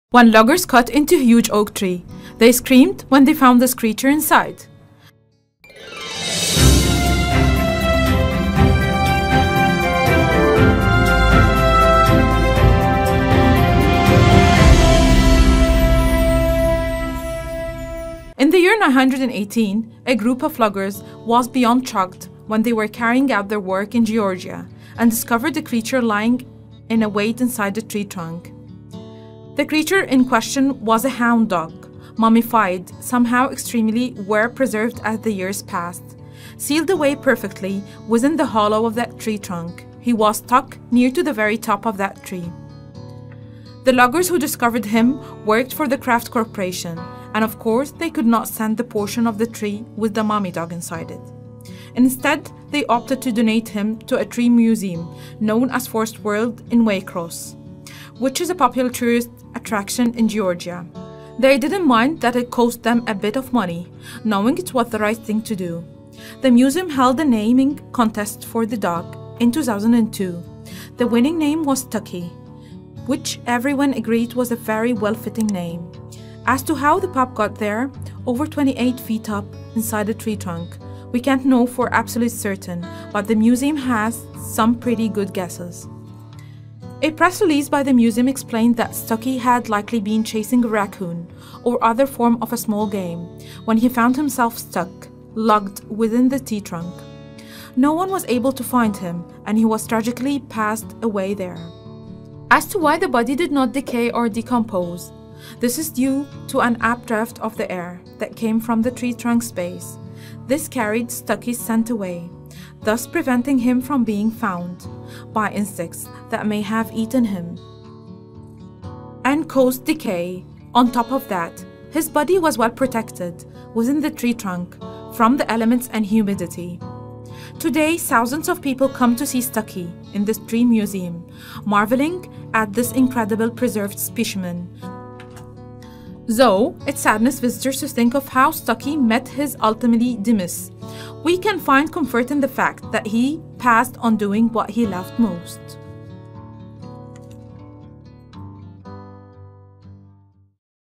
When Loggers Cut Into A Huge Oak Tree, They Screamed When They Found This Creature Inside
In the year 1980, a group of loggers was beyond shocked when they were carrying out their work in Georgia and discovered a creature lying in wait inside a tree trunk.
The creature in question was a hound dog, mummified, somehow extremely well preserved as the years passed, sealed away perfectly within the hollow of that tree trunk. He was stuck near to the very top of that tree.
The loggers who discovered him worked for the Kraft Corporation, and of course, they could not send that portion of the tree with the mummy dog inside it. Instead, they opted to donate him to a tree museum known as Forest World in Waycross, which is a popular tourist attraction in Georgia. They didn’t mind that it cost them a bit of money, knowing it was the right thing to do.
The museum held a naming contest for the dog in 2002, and the winning name was Stuckie, which everyone agreed was a very well-fitting name. As to how the pup got there, over 28 feet up inside a tree trunk, we can’t know for absolute certain – but the museum has some pretty good guesses.
A press release by the museum explained that Stuckie had likely been chasing a raccoon or other form of a small game when he found himself stuck, lodged within the tree trunk. No one was able to find him and he tragically passed away there.
As to why the body did not decay or decompose, this is due to an updraft of air that came from the tree trunk’s base. This carried Stuckie’s scent away, thus preventing him from being found, by insects that may have eaten him and caused decay. On top of that, his body was well protected within the tree trunk from the elements and humidity.
Today, thousands of people come to see Stuckie in the tree museum,marveling at this incredible preserved specimen. Though it saddens visitors to think of how Stuckie met his untimely demise, we can find comfort in the fact that he passed on doing what he loved most.